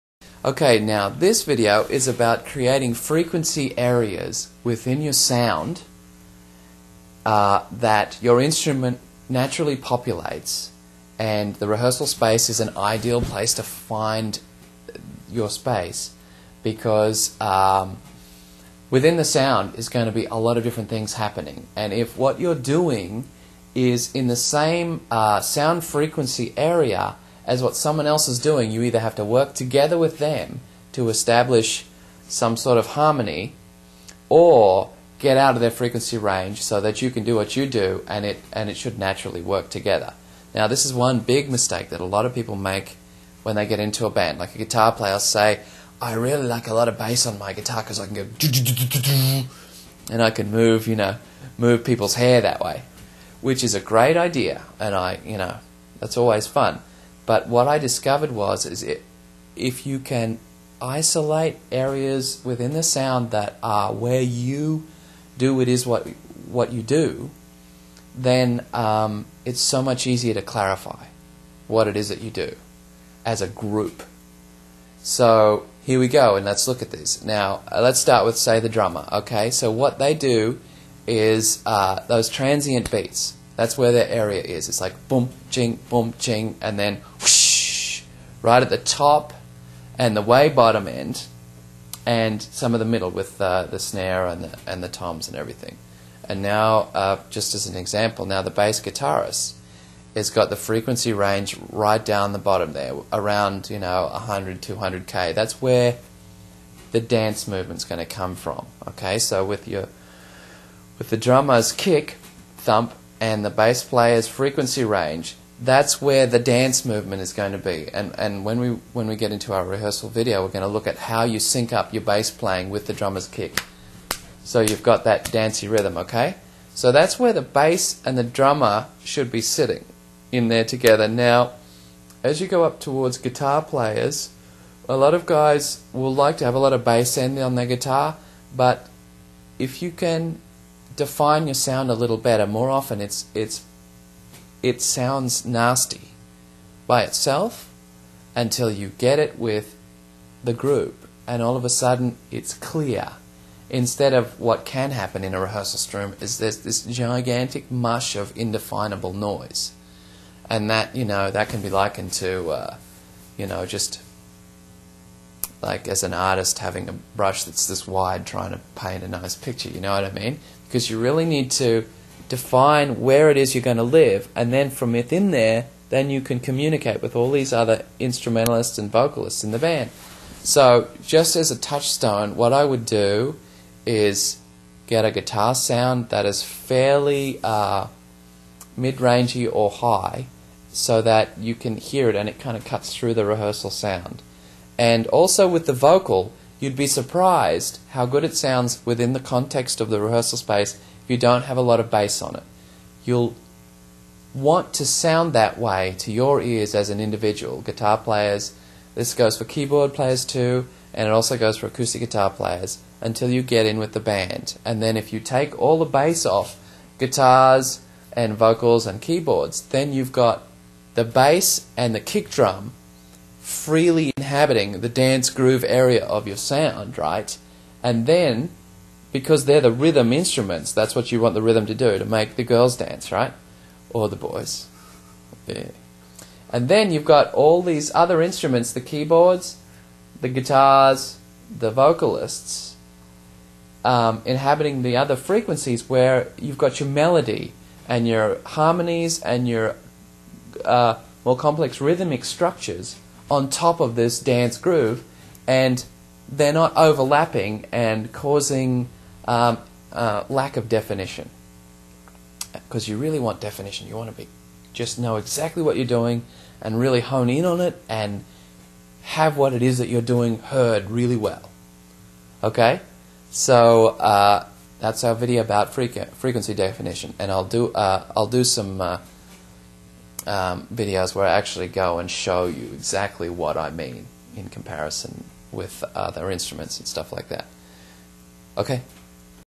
Finding frequency areas within your sound
Cam talks about finding frequency areas within your sound.
A video from the video course "How to start a rock band".